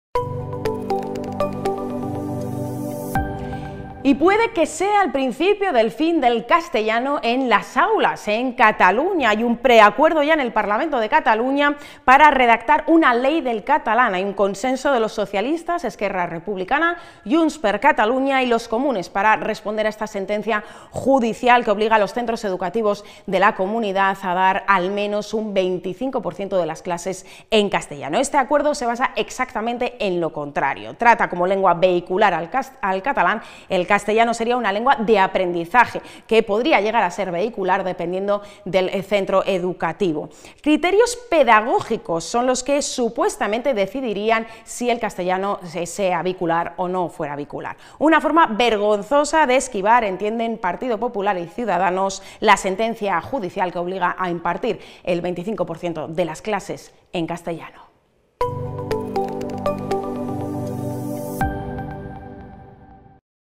El catalán vehicular en las escuelas. Adiós al castellano
Preacuerdo en el parlamento de Cataluña para redactar una ley del catalán. Consenso  de los socialistas, esquerra, junts y los comunes para responder a esa sentencia judicial que obliga a los centros educativos de la comunidad a impartir, al menos, un 25% de las clases en castellano. Este acuerdo se basa en exactamente lo contrario. Trata como lengua vehicular al catalán. El castellano sería una lengua de aprendizaje que podría llegar a ser vehicular, eso sí, dependiendo del centro educativo. Criterios pedagógicos son los que, supuestamente, decidirían si se habla español o no en las clases. Una forma vergonzosa de esquivar, entienden el PP y CS, la sentencia judicial que obliga a impartir al menos el 25% de las clases en castellano.